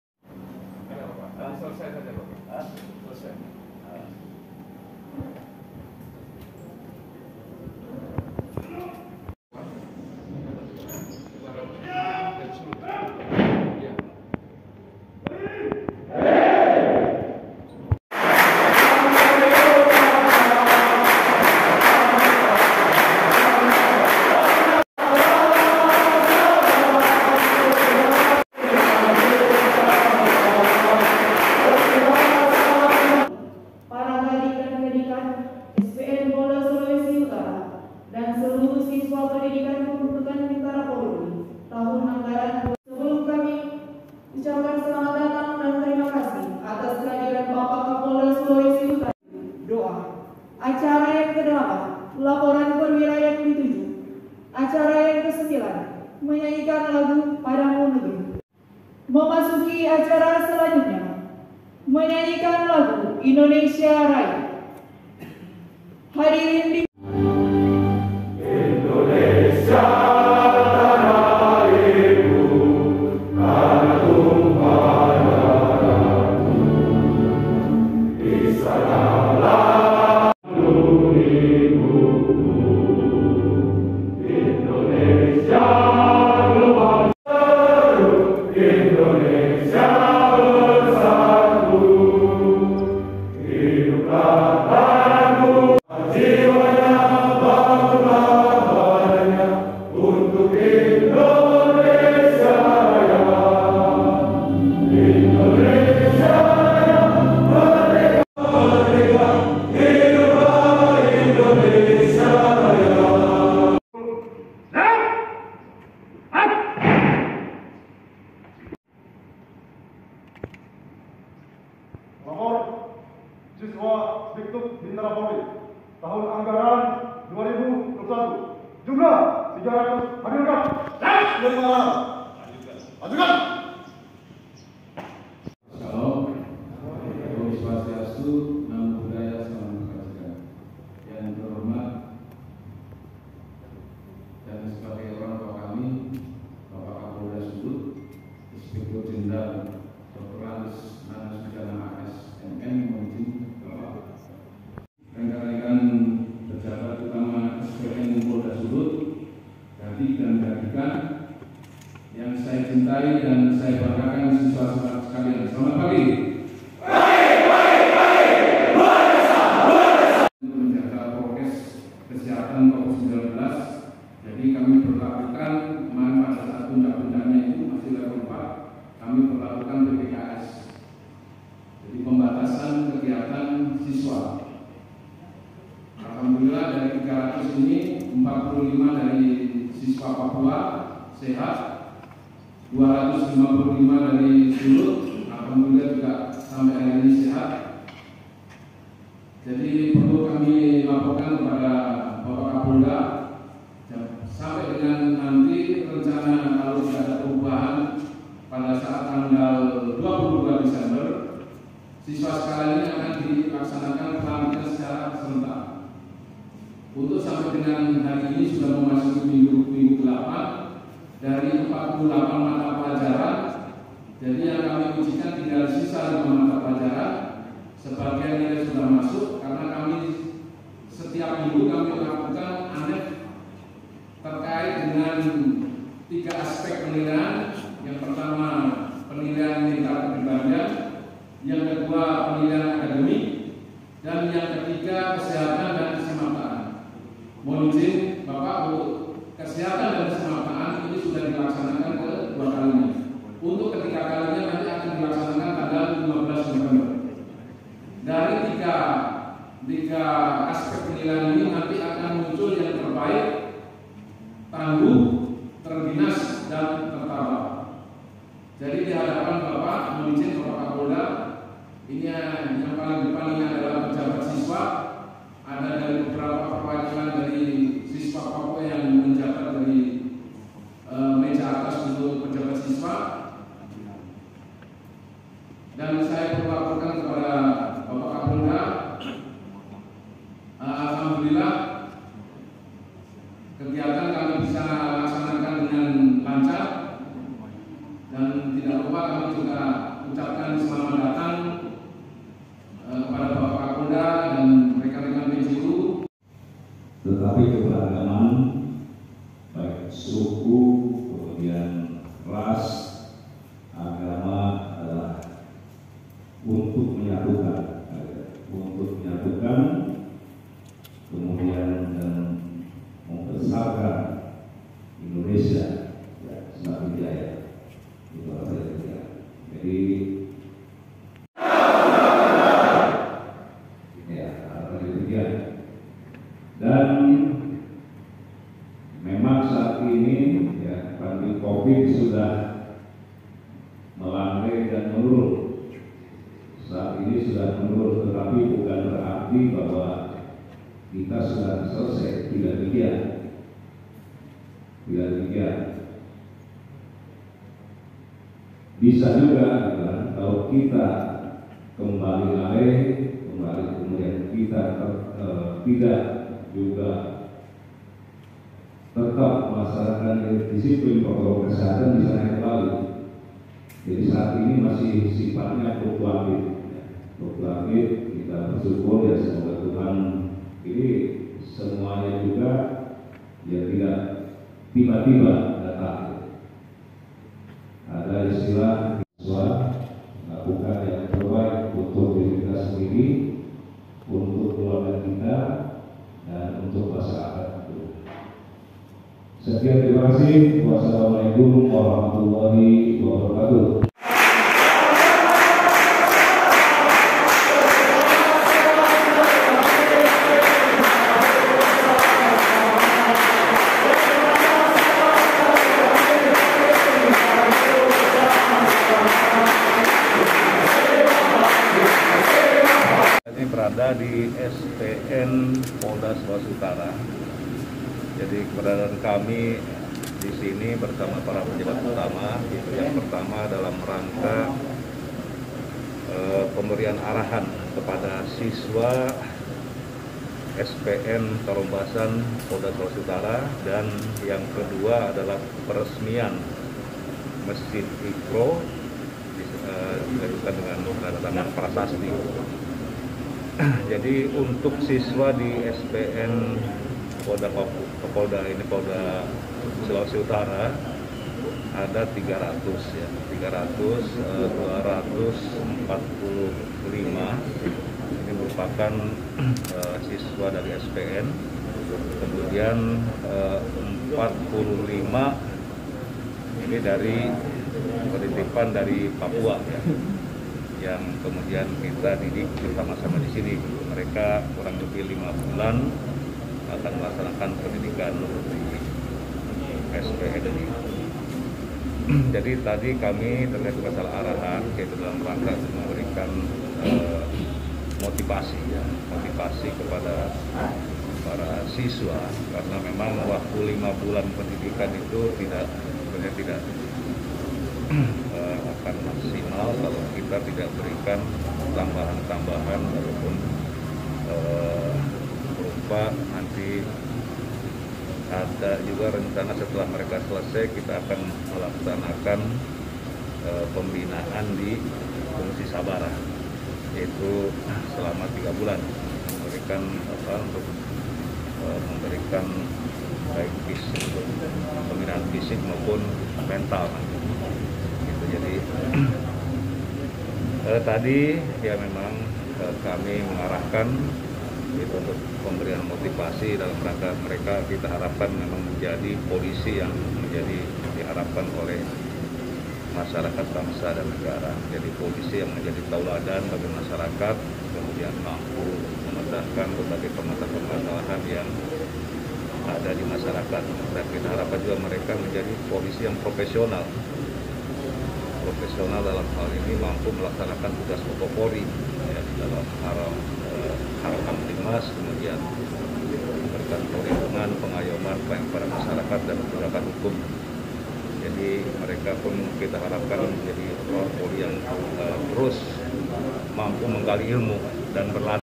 Seperti Ini Arahan Kapolda pada 300 Siswa Diktukba Polri di SPN Polda Sulut
Kapolda Sulawesi Utara Irjen Pol Nana Sudjana memberikan pembekalan dan arahan kepada 300 Siswa Diktukba Polri yang sedang melaksanakan pendidikan di SPN Polda Sulut.

Pembekalan dan arahan ini digelar di Graha Dotulong SPN Polda Sulut, Kamis (4/11/2021), yang juga dihadiri oleh beberapa PJU Polda, Kepala SPN Polda Sulut dan seluruh pejabat SPN.

Kapolda Irjen Pol Nana Sudjana mengajak seluruh siswa agar terus bersyukur kepada Tuhan karena masih diberikan kesehatan dan masih tetap menjadi bagian dari keberagaman Indonesia, di tengah situasi pandemi covid-19.

“Kita harapkan para siswa ini menjadi polisi yang bisa menjadi harapan masyarakat, bangsa dan negara, polisi yang menjadi teladan bagi masyarkat untuk memecahkan berbagai permasalahan yang ada di tengah masyarakat,” ujar Irjen Pol Nana Sudjana.

Para siswa yang akan dilantik menjadi anggota Kepolisian berpangkat Bripda pada tanggal 22 Desember 2021 ini juga diharapkan dapat menjadi polisi yang profesional, yang mampu melaksanakan tugas pokok Polri, dalam rangka harkamtibmas, perlindungan, pengayoman dan pelayanan masyarakat serta penegakan hukum.

Dalam kunjungannya ke SPN Polda Sulut, Irjen Pol Nana Sudjana juga melakukan peresmian renovasi Masjid Iqro yang berada di dalam kompleks SPN Polda Sulut. Peresmian ditandai dengan penandatanganan prasasti dilanjutkan dengan pengguntingan pita oleh Kapolda.